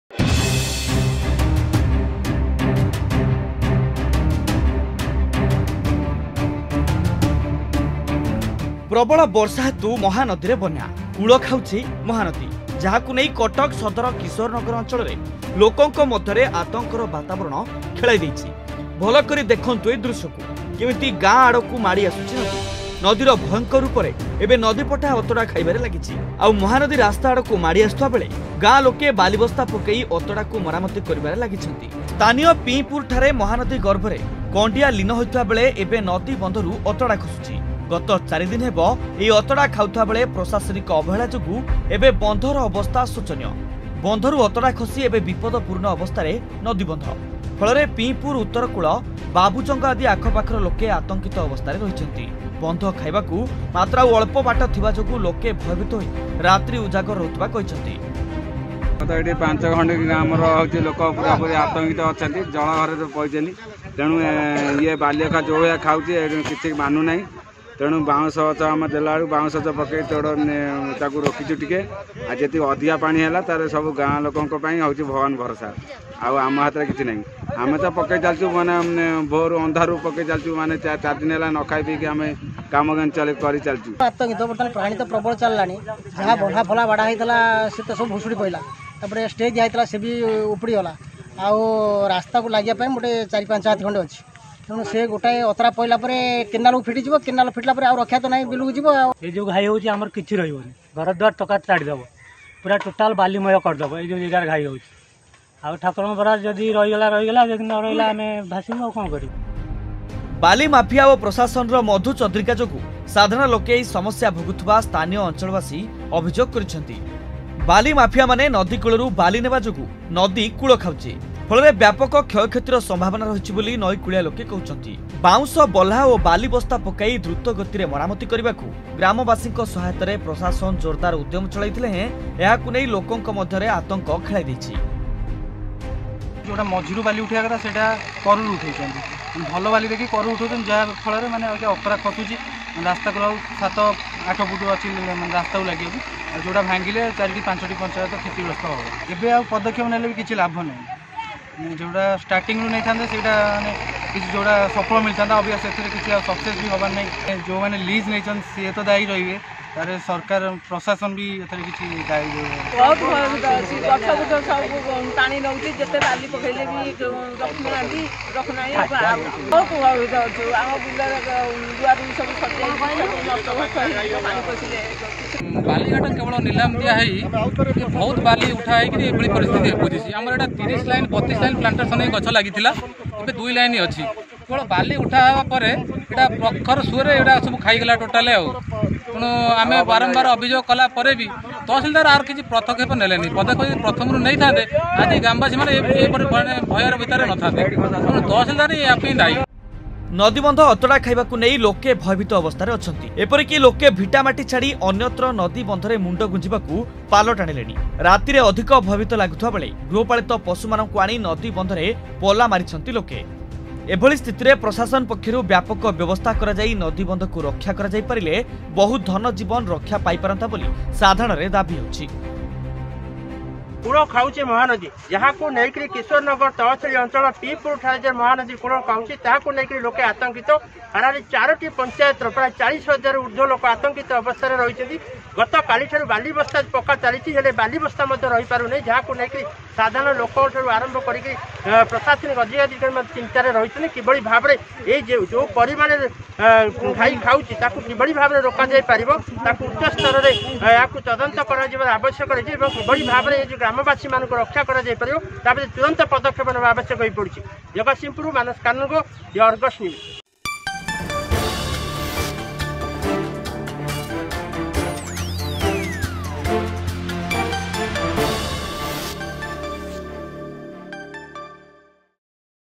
କୂଳ ଖାଉଛି ମହାନଦୀ
କୂଳ ଖାଉଛି ମହାନଦୀ । ଜନବସତି ଆଡକୁ ମାଡି ଆସିବାରେ ଲାଗିଲାଣୀ ନଦୀ । ଯାହାକୁ ନେଇ ଲୋକମାନଙ୍କ ମଧ୍ୟରେ ଭୟର ବାତାବରଣ ଖେଳାଇ ଦେଇଛି । କଟକ ସଦର କିଶୋର ନଗର ତହସିଲ ପିଇଁପୁର ଠାରେ କୁଳ ଖାଉଛି ମହାନଦୀ